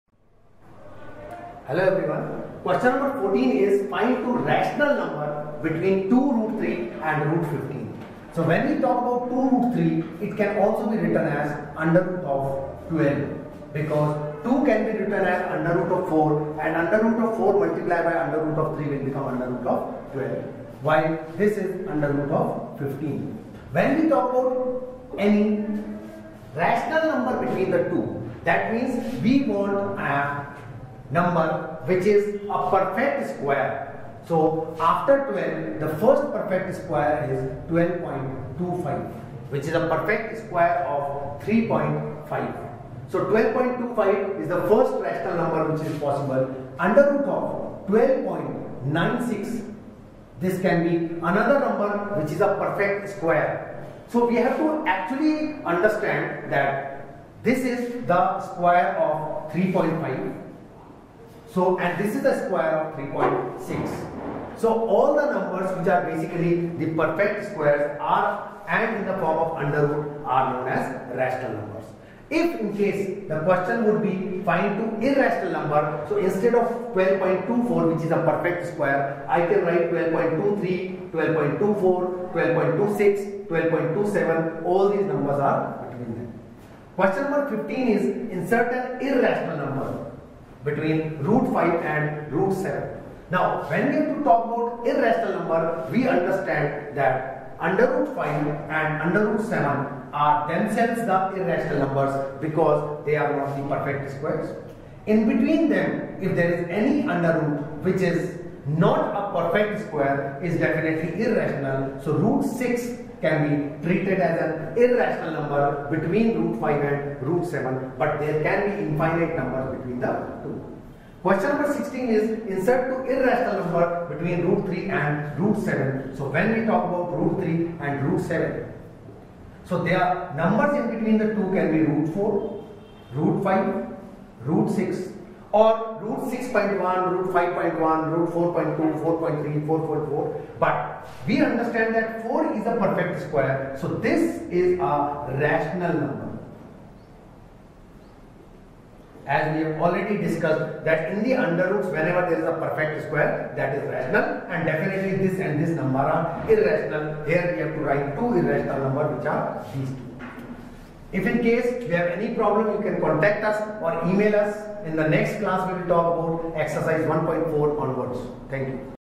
Exercise 1.3 (Q14, Q15) Solution for Class 9th Understanding ICSE Mathematics by ML Aggarwal
In this video we are solving class 9th Understanding ICSE Mathematics Book by ML Aggarwal - Exercise 1.3 from Question number 14 to Question number 16.